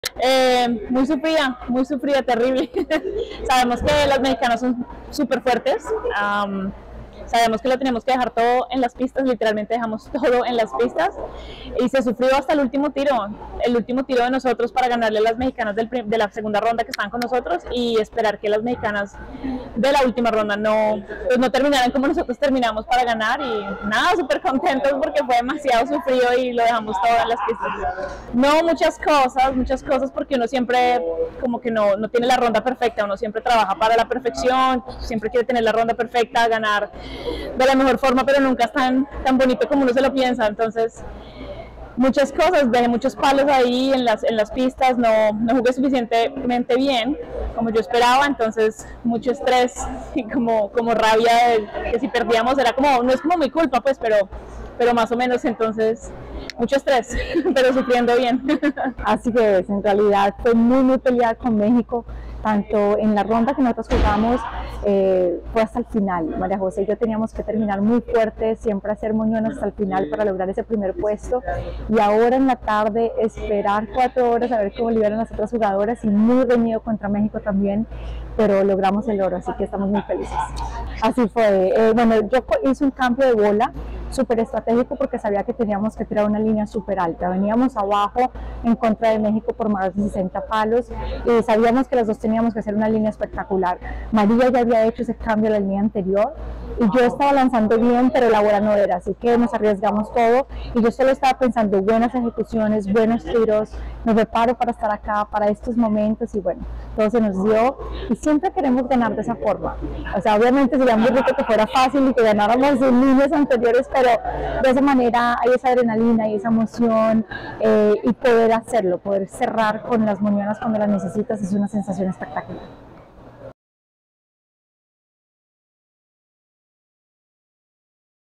María Rodríguez y Clara Guerrero - Oro dobles femenino - Bowling
Las bolicheras colombianas Clara Juliana Guerrero y María José Rodríguez cerraron la jornada con una medalla de oro en la prueba de dobles femeninos desde el Villa Cari, se enfrentaron en la final al equipo mexicano. Con un puntaje total de 2613, Guerrero y Rodríguez se aseguraron la medalla de oro, superando por tan solo cinco puntos a la dupla mexicana conformada por Ilana Lomeli y Aseret Zetter, quienes se llevaron la plata con 2608 puntos. El bronce fue para Sandra Gongora y Graciela Sánchez de México, quienes obtuvieron 2607 puntos.